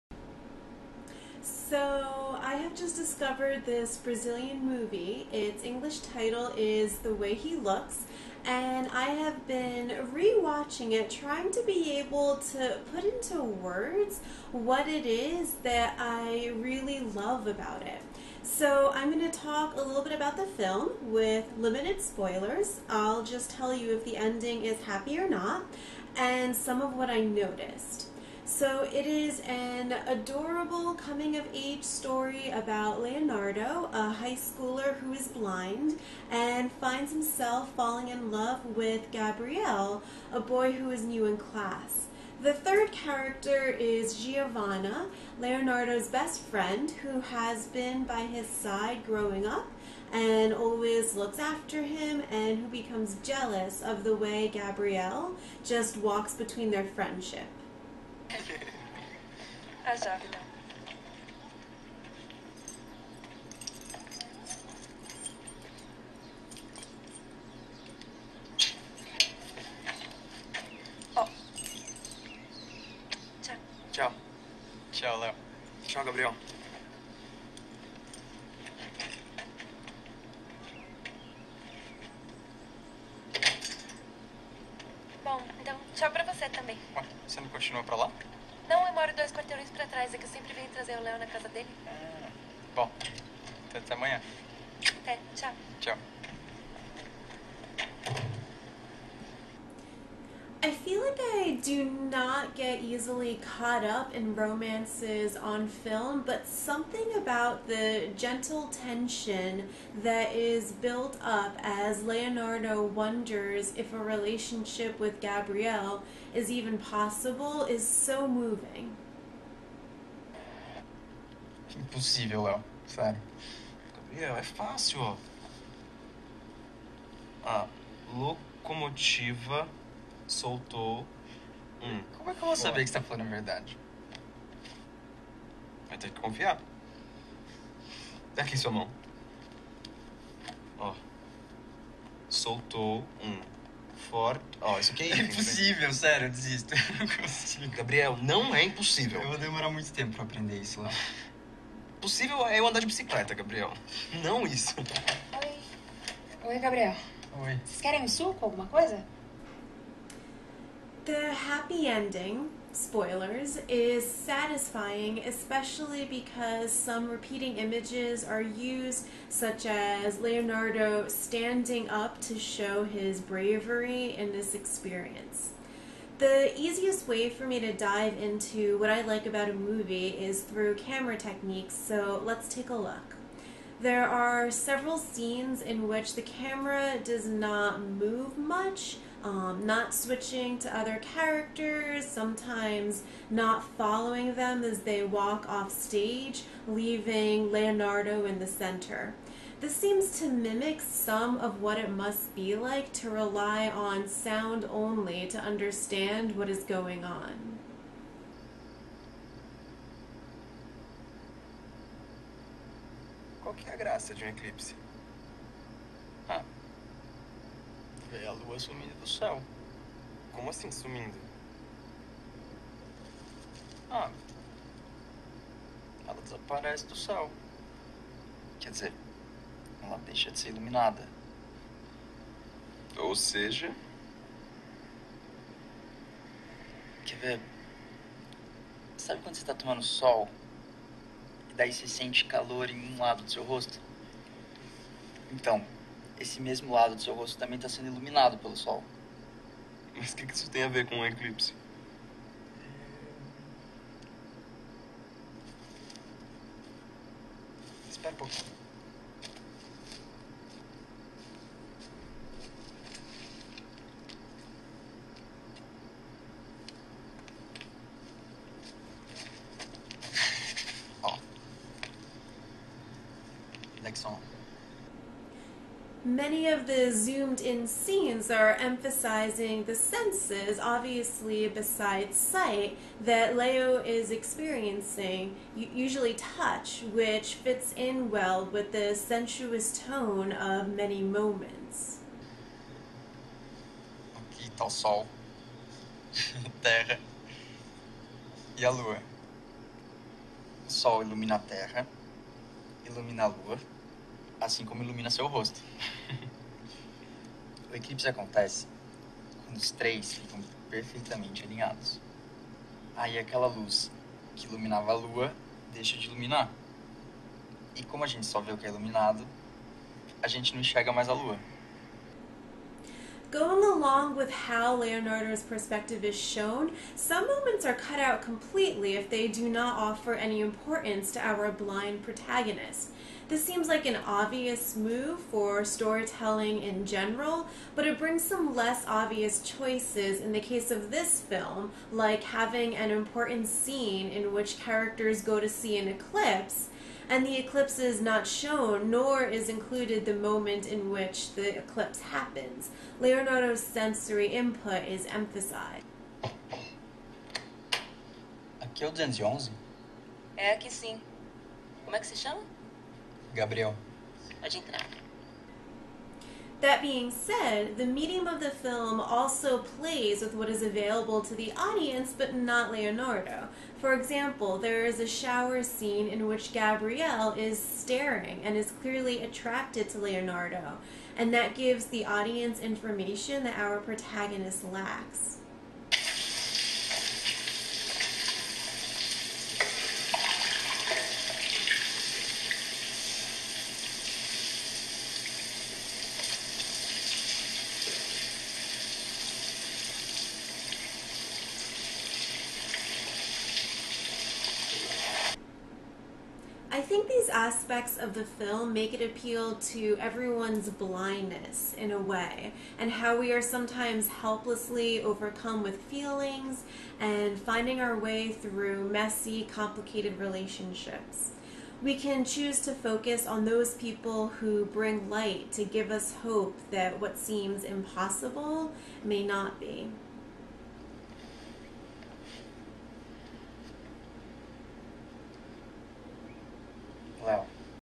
The Way He Looks Review
Reaction & analysis of the Brazilian film, "The Way He Looks."  Based on short film "I Don't Want To Go Back Alone"(Portuguese: "Hoje Eu Quero Sozinho")
(Film Information: Director Daniel Ribeiro, Starring Ghilherme Lobo, Fabio Audi, Tess Amorim, 2014)